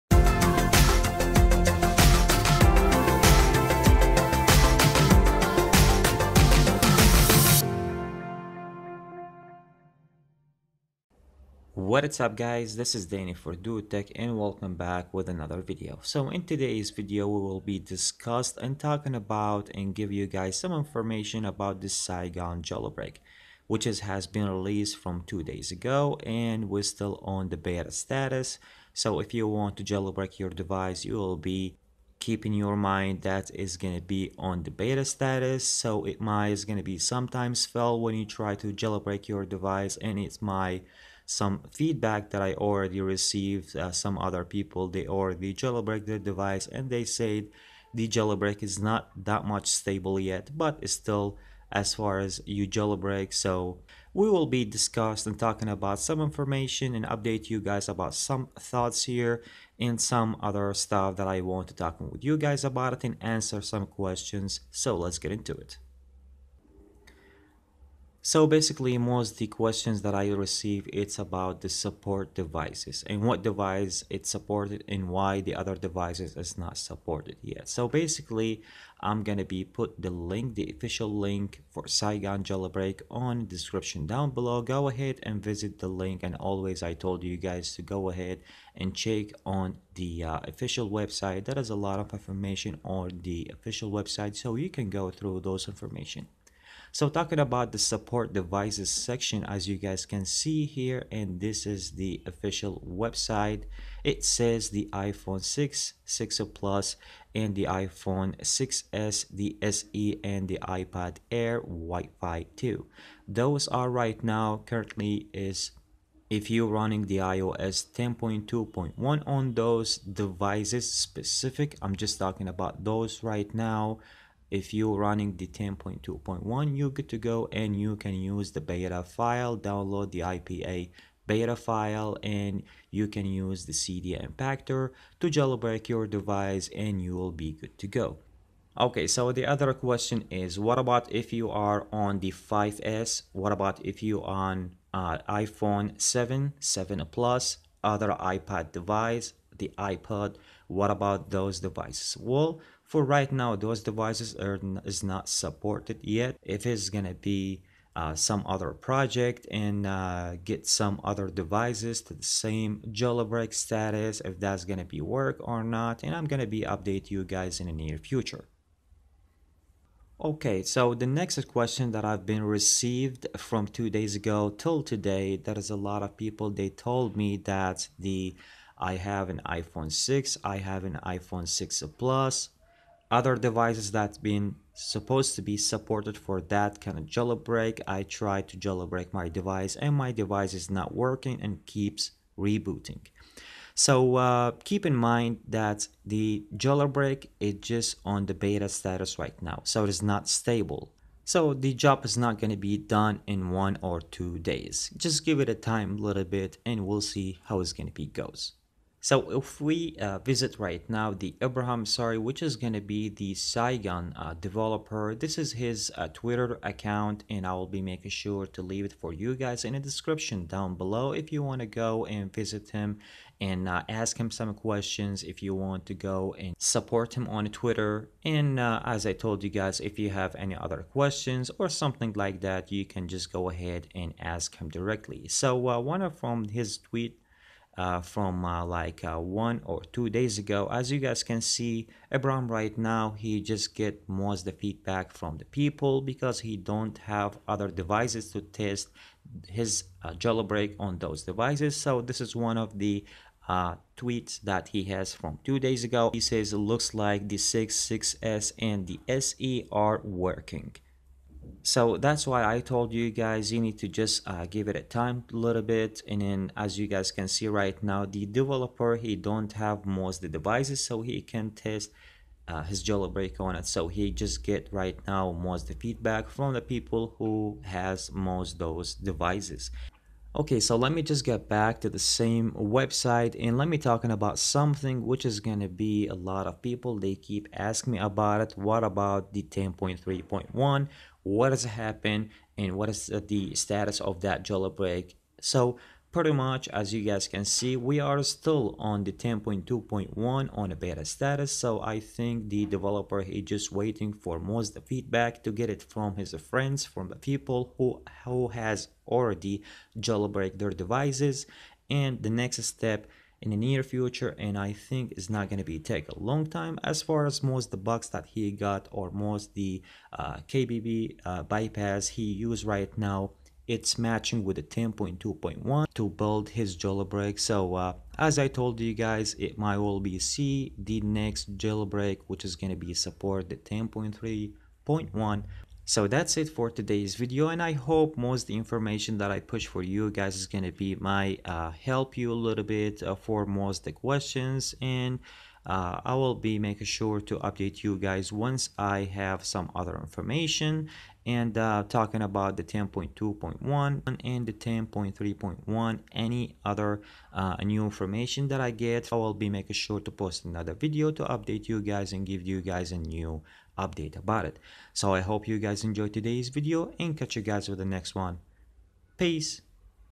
iOS 10.2.1 / 10.3.1 / 10.3.2 SAIGON Jailbreak Status And Update
Hi Guys! In today's video i have new information and update status for  Saigon jailbreak !!